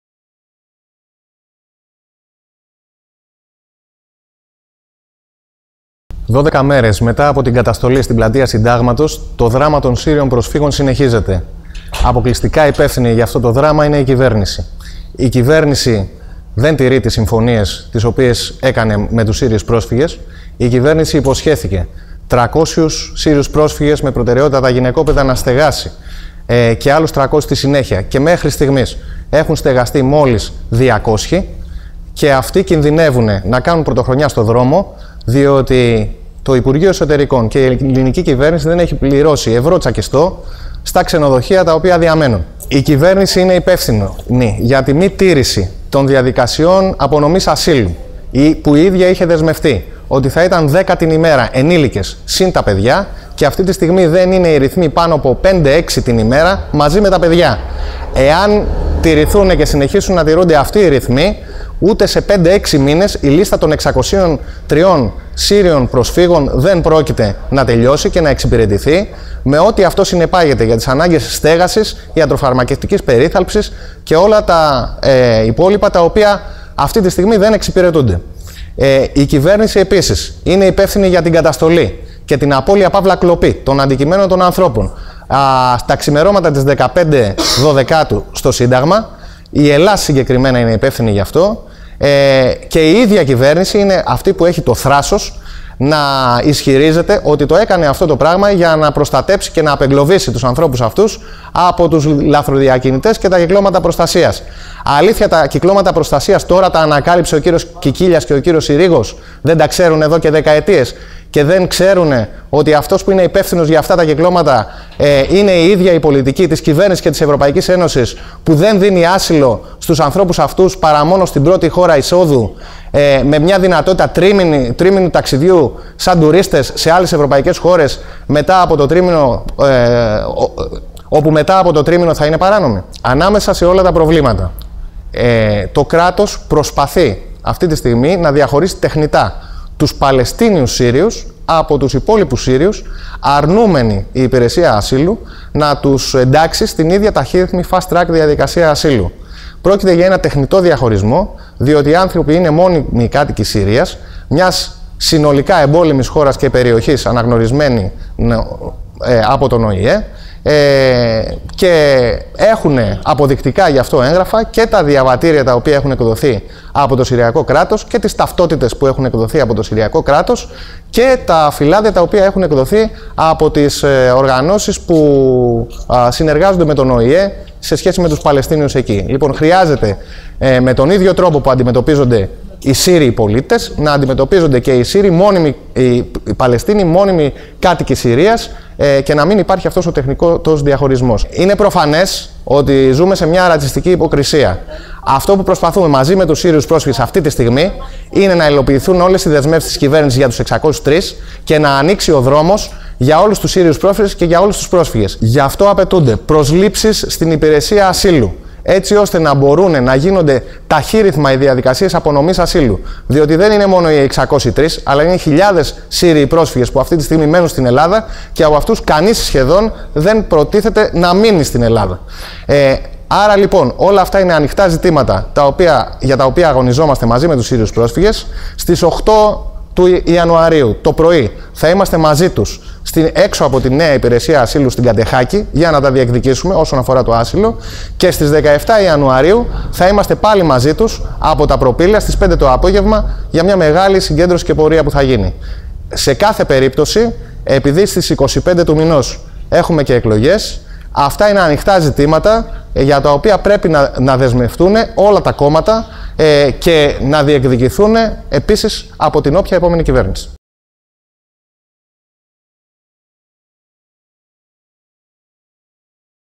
Ο αγώνας των Σύρων προσφύγων συνεχίζεται -Θανάσης Κούρκουλας
Ο αγώνας των Σύρων προσφύγων συνεχίζεται
Συνέντευξη Τύπου δόθηκε την Τρίτη 30/12 στο στέκι μεταναστών για τις εξελίξεις του αγώνα των Σύρων προσφύγων του Συντάγματος από τους ίδιους τους Σύρους και τη Συνέλευση Αλληλεγγύης στους Σύρους Πρόσφυγες. Στην συνέντευξη Τύπου παραβρέθηκε εκ μέρους της Υπηρεσίας Ασύλου -Περιφεριακό Γραφείο Ασύλου Αττικής, η κυρία Ελένη Πετράκη.
Δώδεκα μέρες μετά την αστυνομική καταστολή στην πλατεία Συντάγματος κανένα από τα αιτήματα των Σύρων προσφύγων πολέμου δεν έχει ικανοποιηθεί. Η κυβέρνηση είχε υποσχεθεί αλλά δεν τήρησε:
- Να στεγαστούν άμεσα 300 πρόσφυγες με προτεραιότητα στα γυναικόπαιδα και άλλους 300 στη συνέχεια. Μέχρι στιγμής έχουν στεγαστεί μόλις 200 και αυτοί κινδυνεύουν να βρεθούν την πρωτοχρονιά στο δρόμο γιατί η κυβέρνηση δεν έχει πληρώσει τα ξενοδοχεία στα οποία διανέμουν.
 - Οι διαδικασίες απονομής ασύλου και ταξιδιωτικών εγγράφων προχωρούν με ρυθμούς χελώνας και όχι ταχύρυθμα όπως είχε υποσχεθεί η κυβέρνηση. Το αποτέλεσμα είναι με τους ρυθμούς αυτούς να απαιτούνται πολλοί μήνες αναμονής για τους πρόσφυγες που έχουν εξαντληθεί τόσο φυσικά όσο και οικονομικά.
-Επιπλέον, το κράτος διαχωρίζει τεχνικά τους Παλαιστίνιους Σύρους από τους υπόλοιπους Σύρους, αρνούμενη η υπηρεσία ασύλου του ελληνικού κράτους να τους εντάξει στη ίδια ταχύρυθμη διαδικασία ασύλου όπως τους άλλους Σύρους πρόσφυγες.
Η Συνέλευση Αλληλεγγύη στους Σύρους Πρόσφυγες σε συνεργασία με τους πρόσφυγες διοργανώνει στις 8 Ιανουαρίου συγκέντρωση-διαμαρτυρία έξω από τη νέα υπηρεσία ασύλου στην οδό Κατεχάκη (όπου βρίσκεται το περιφερειακό γραφείο Αττικής). Στις 17 Ιανουαρίου θα γίνει μεγάλη διαδήλωση και πορεία στα Προπύλαια στην Αθήνα.
Στο φακό του left και τον Άγγελος Καλοδούκα έκαναν μικρές δηλώσεις οι Θανάσης Κούρκουλας, μέλος της Κίνησης «Απελάστε τον Ρατσισμό», η Βασιλική Κατριβάνου, βουλευτής ΣΥΡΙΖΑ και ο Γιάννης Ανδρουλιδάκης, μέλος της Συνέλευσης Αλληλεγγύης στους Σύρους Πρόσφυγες.